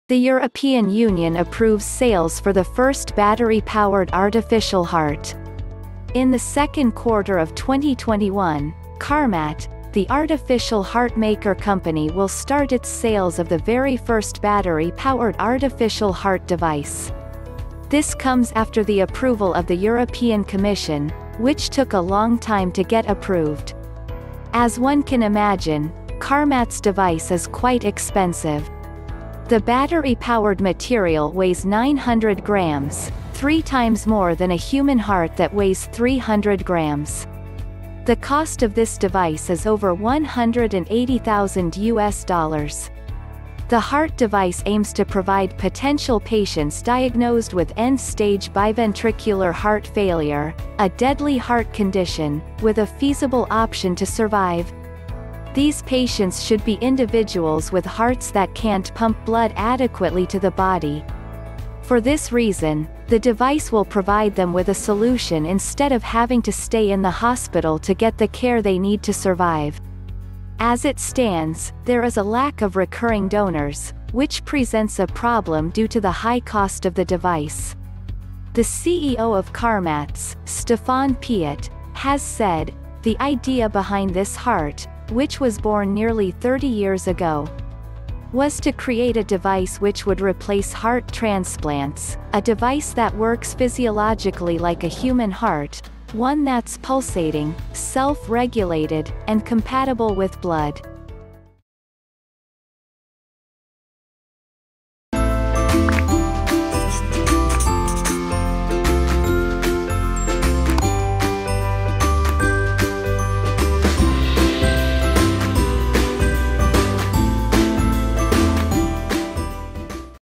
EU Approves Sales for the 1st Battery Powered, Carmat Artificial Heart
The EU approves sales for the first battery powered artificial heart, by Carmat, the artificial heart-maker company. Carmat Artificial Heart. NewsTechTV

View More Medical Tech Videos Here: ​

The EU Approves Sales for the First Battery-Powered Artificial Heart

In the second quarter of 2021, Carmat, the artificial heart-maker company will start its sales of the very first battery-powered artificial heart device. This comes after the approval of the European Commission, which took a long time to get approved.

As one can imagine, Carmat’s device is quite expensive. The battery-powered material weighs 900 grams, three times more than a human heart that weighs 300 grams. The cost of this device is over $180,000.

The heart device aims to provide potential patients diagnosed with end-stage biventricular heart failure, a deadly heart condition, with a feasible option to survive. These patients should be individuals with hearts that can't pump blood adequately to the body. For this reason, the device will provide them with a solution instead of having to stay in the hospital to get the care they need to survive. As it stands, there is a lack of recurring donors, which presents a problem due to the high cost of the device.

The CEO of Carmat’s, Stephane Piat, has said, "The idea behind this heart, which was born nearly 30 years ago, was to create a device which would replace heart transplants, a device that works physiologically like a human heart, one that's pulsating, self-regulated, and compatible with blood."
Piat continued by saying that the company had a goal to secure their extension of an EU approval to cater to anyone that, due to pre-existing conditions, can't wait to be considered on a month-to-month or even year-to-year waiting list for heart transplants. This solution is for anyone that needs a heart fast and can't wait for an extensive period to get a heart on a donor list.